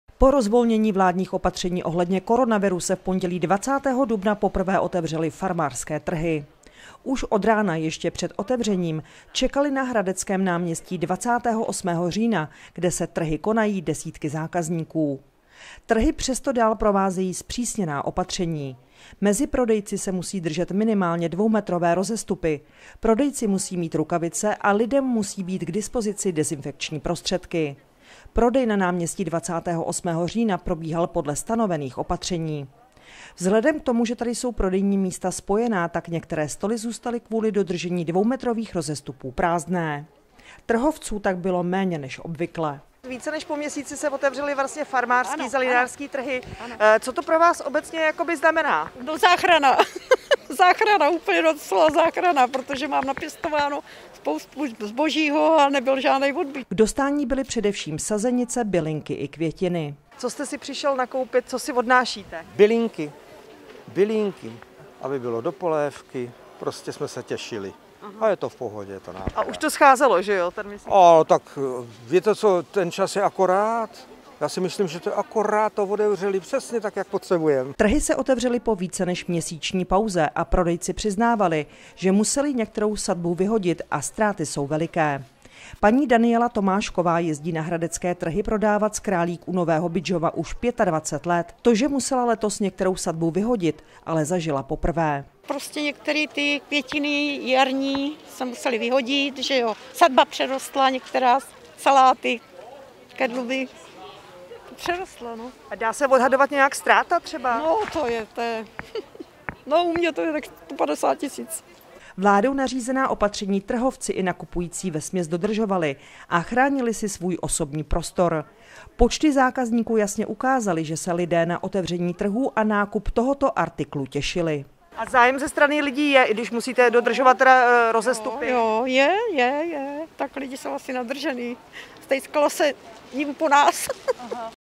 Trhy se otevřely, zájem je veliký
Po rozvolnění vládních opatření ohledně koronaviru se v pondělí 20. dubna poprvé otevřely farmářské trhy. Už od rána – ještě před otevřením - čekaly na hradeckém náměstí 28. října, kde se trhy konají, desítky zákazníků. Trhy přesto dál provázejí zpřísněná opatření. Mezi prodejci se musí držet minimálně dvoumetrové rozestupy, prodejci musí mít rukavice a lidem musí být k dispozici dezinfekční prostředky. Prodej na náměstí 28. října probíhal podle stanovených opatření. Vzhledem k tomu, že tady jsou prodejní místa spojená, tak některé stoly zůstaly kvůli dodržení dvoumetrových rozestupů prázdná. Trhovců tak bylo méně než obvykle.